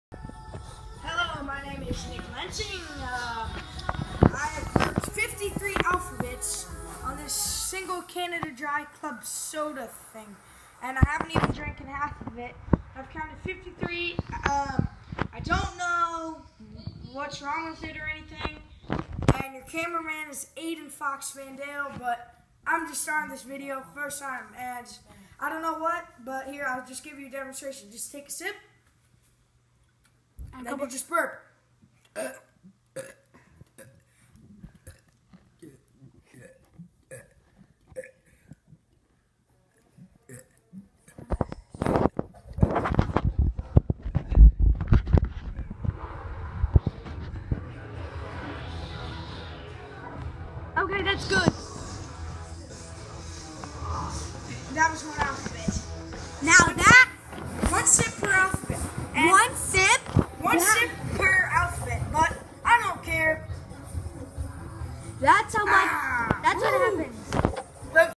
Nick burps like crazy!
Drinks club soda... Uncontrollable burping